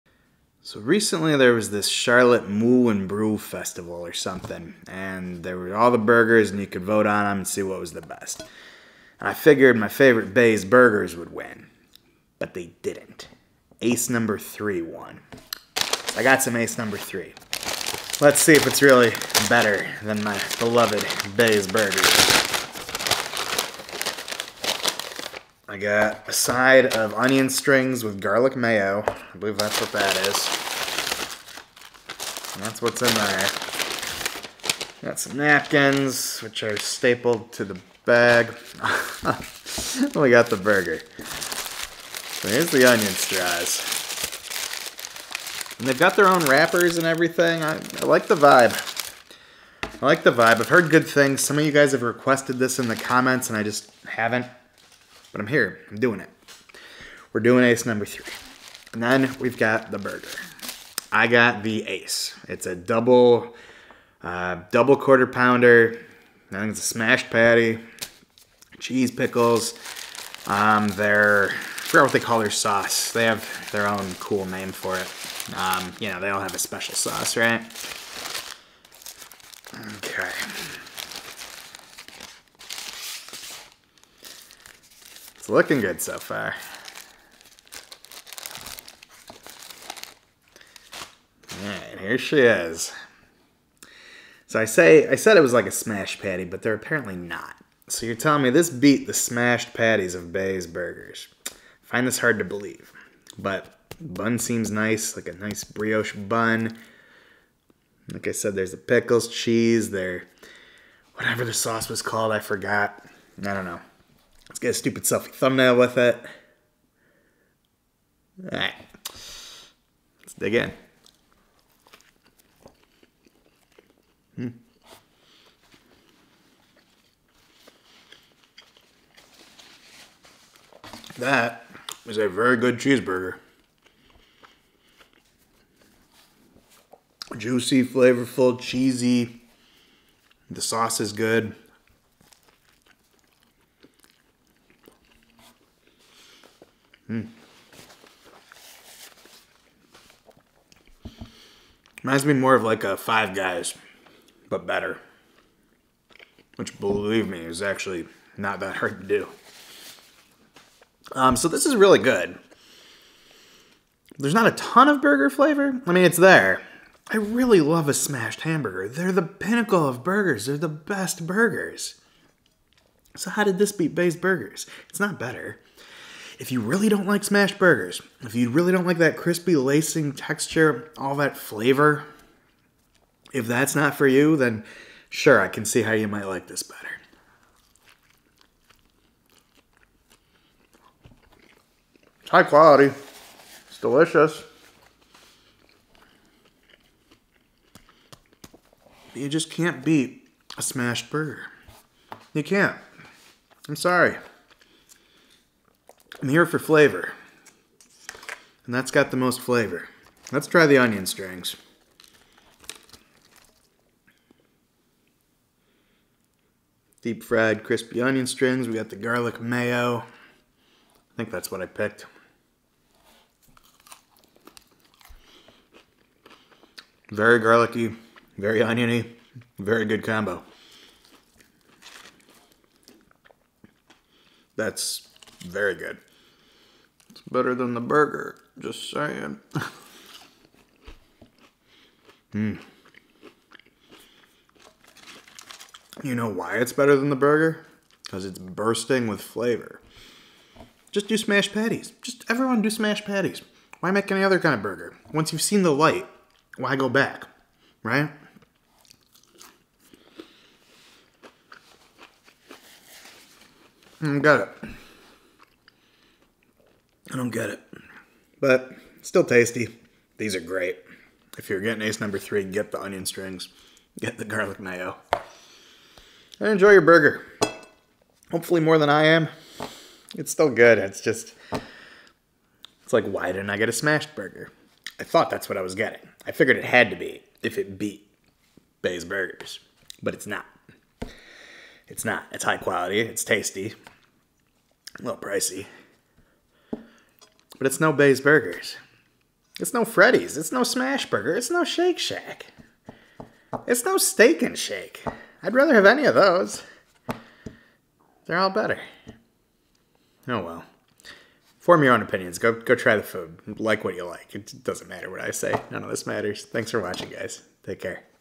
Ace No. 3 Cheeseburger Review - Charlotte, NC
A while back I tried out local Charlotte, NC favorite Ace No. 3 and filmed it. Dug up the footage today for y'all to check out.